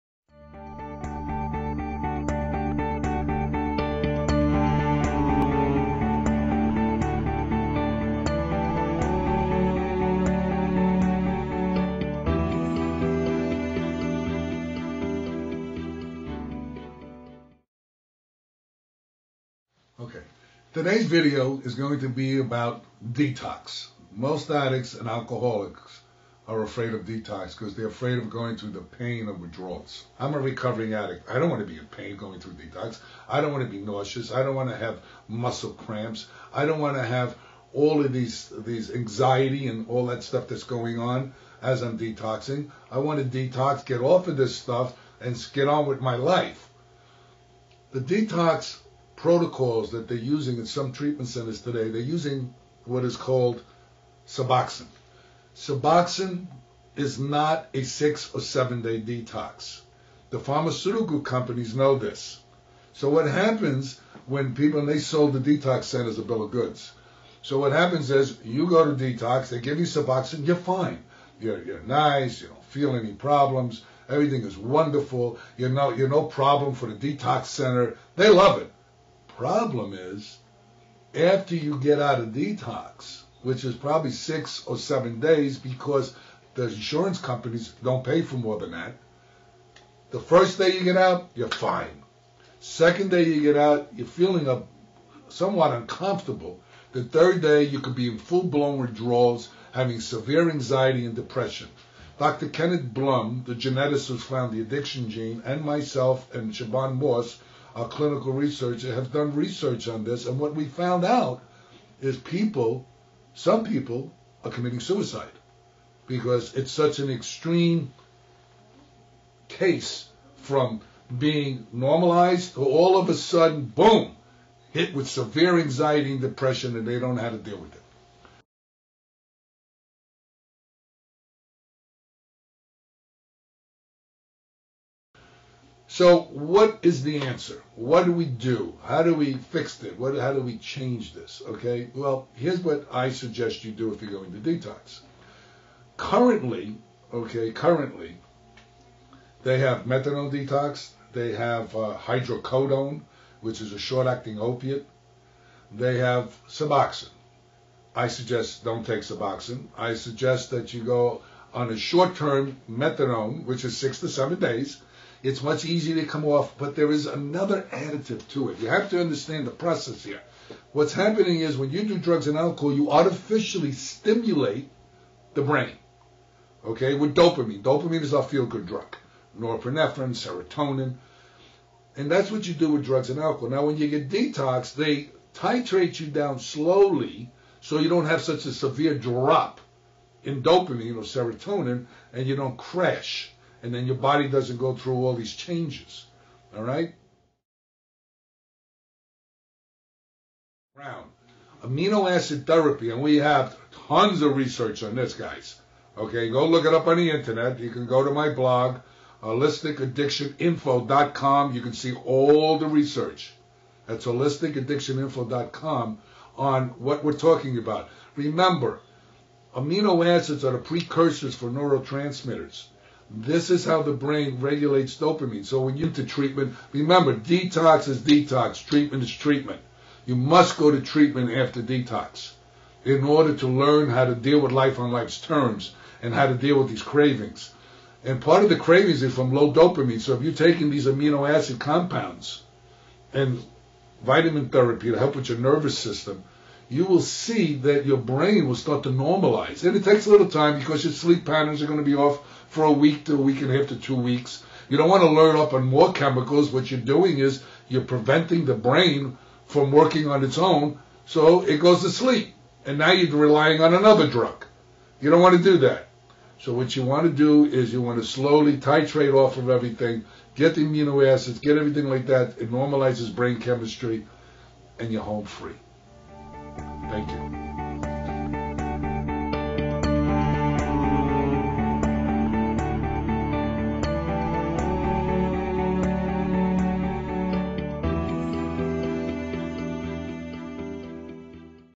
John Giordano Correct Oxycodone Detox
- Correct Oxycodone Detox - John Giordano of G&G Holistic Drug Treatment Center discusses the correct way to detox from Oxycodone. Our addiction treatment services are tailored to your individual needs.